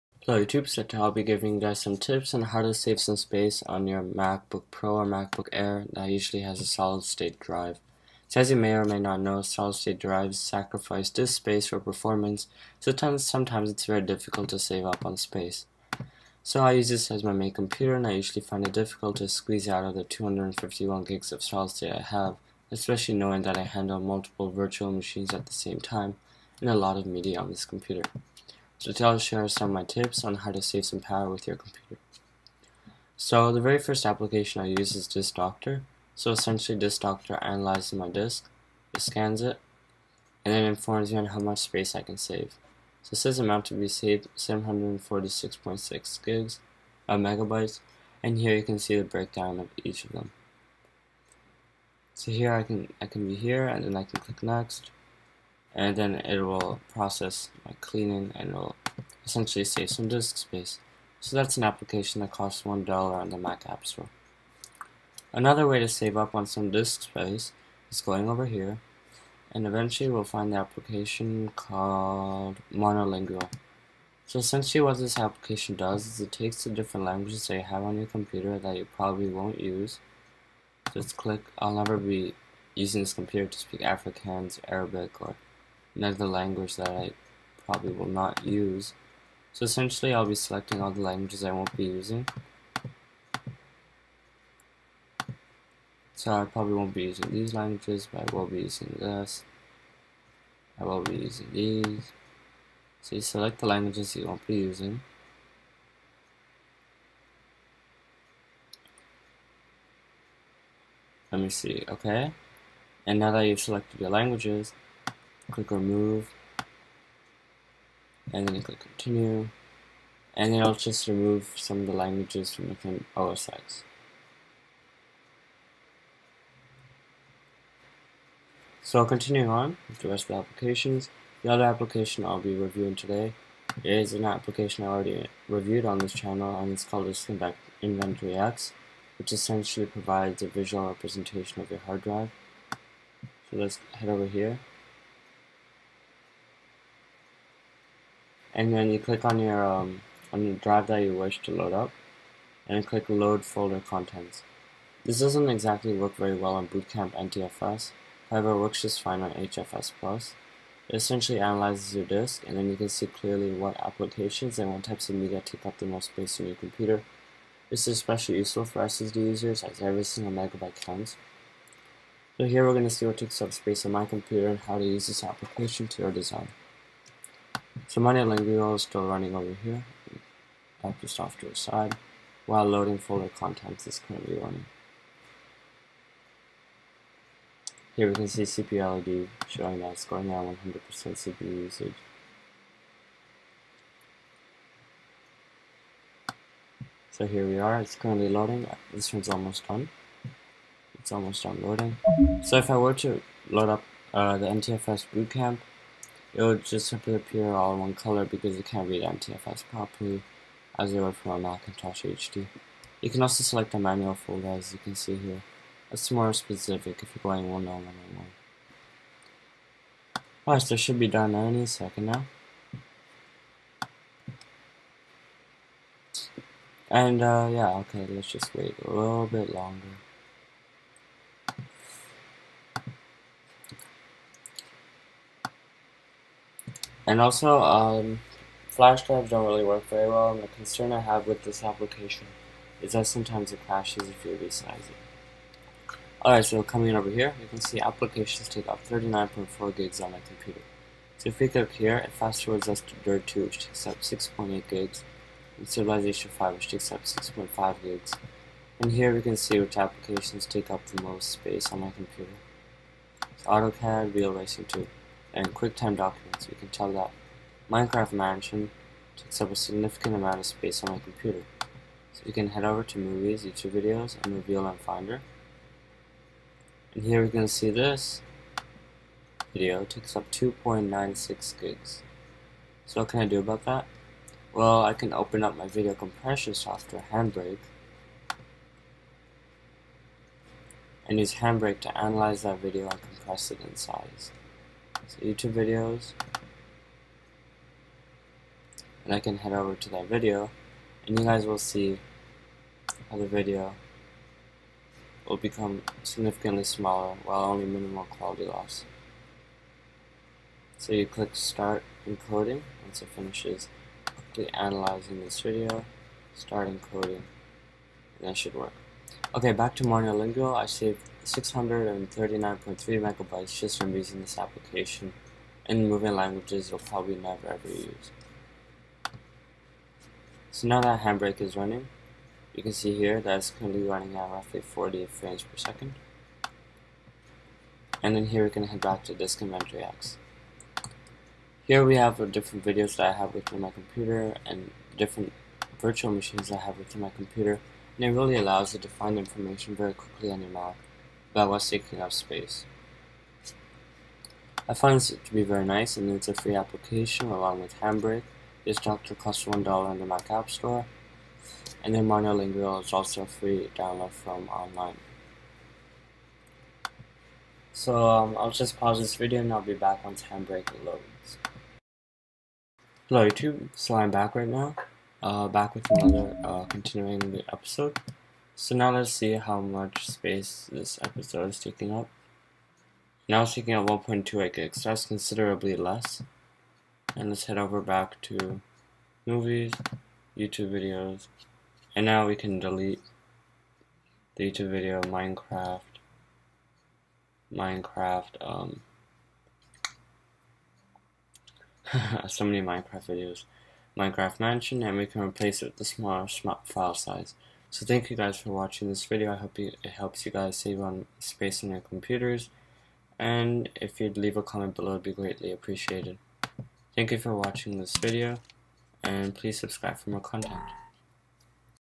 How To: Save Space on a Mac! (2013 / 2014 UPDATED)
Learn how to save space on a Mac by removing foreign languages, and quickly finding what takes up the most space on your computer. Follow this tutorial and your SSD will be full of space for you to use. :)

Learn how to compress videos using handbreak, use Disk Inventory X and remove languages. :)